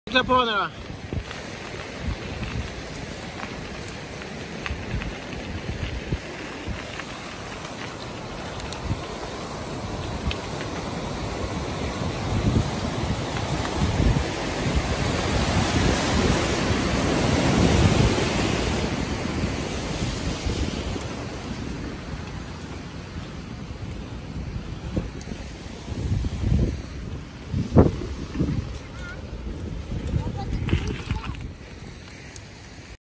Mtb tur med sonen i Hallstahammar
Sonen 6år på sin
ORBEA MX Trail 24"